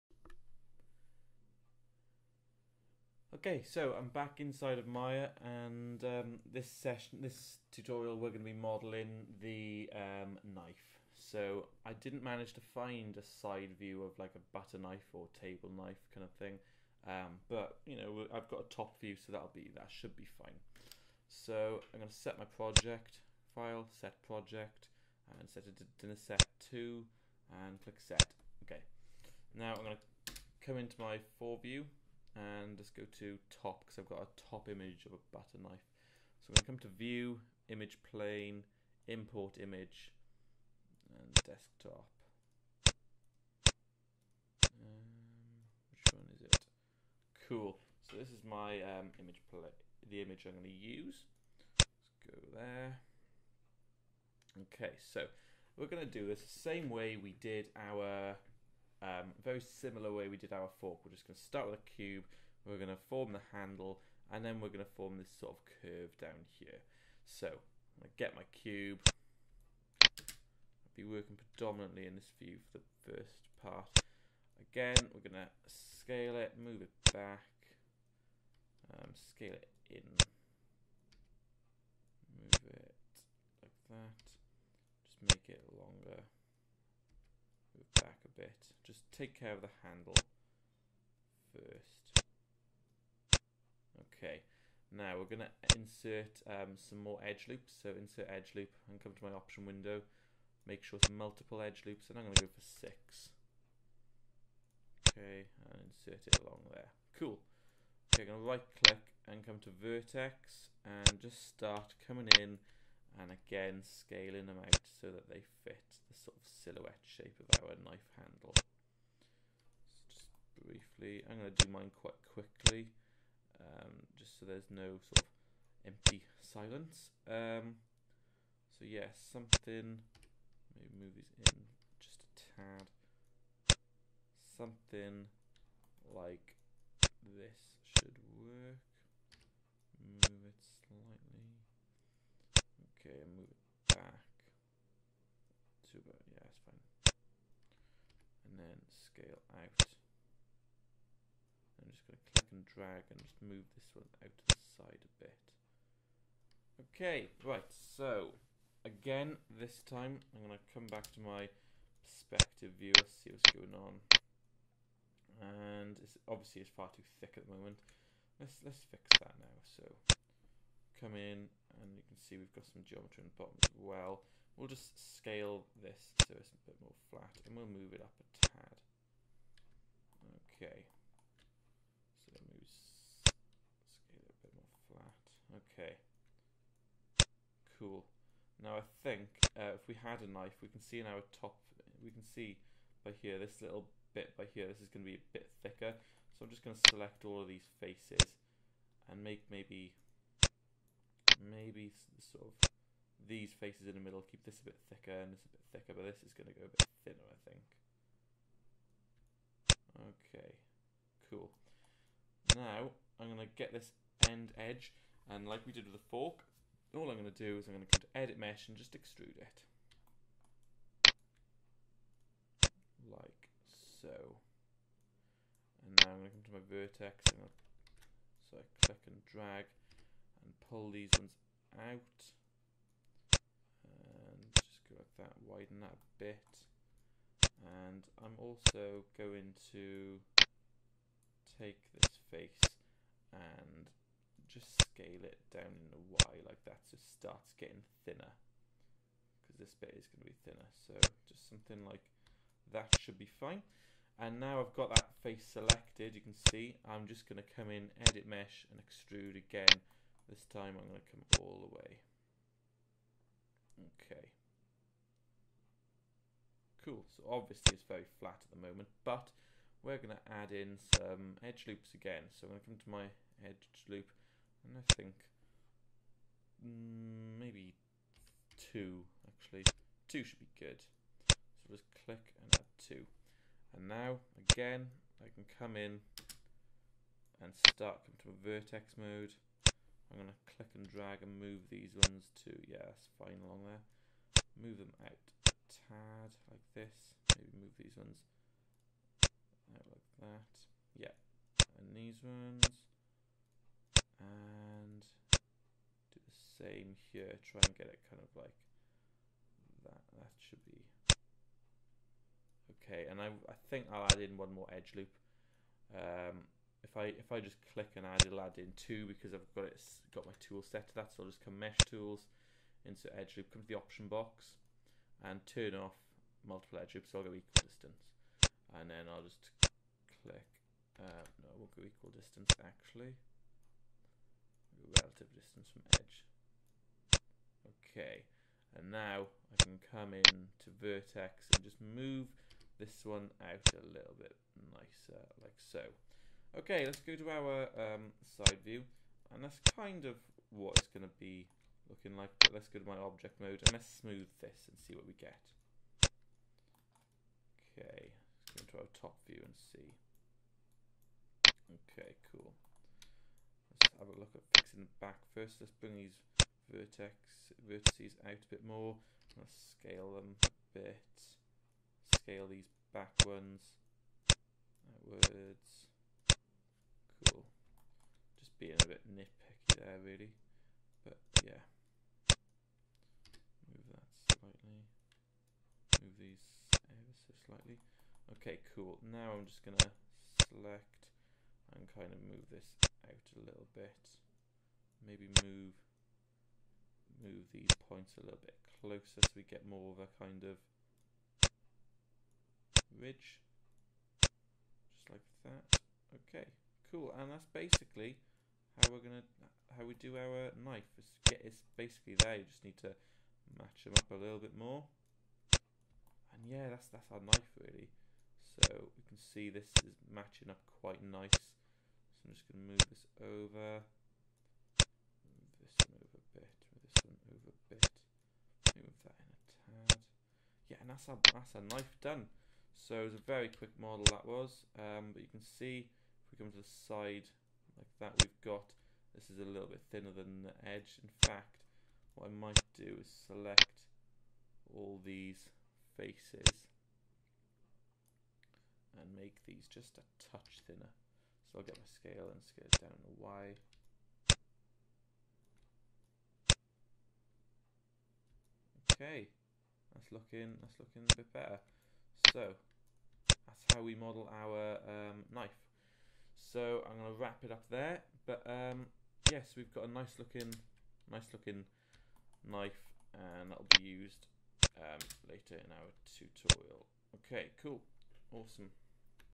Modelling a Knife in Maya
In thit tutorial I will show you how to model a Knife in Maya